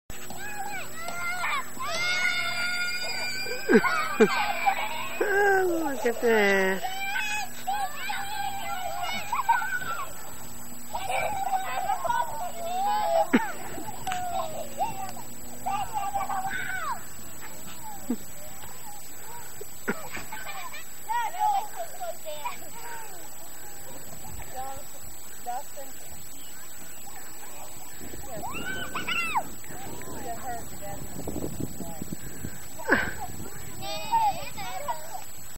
Kids spinning on Merry-go-round at Hough Park-2006
Video recorded at Mannington Hough Park in Mannington, WV May 20, 2006.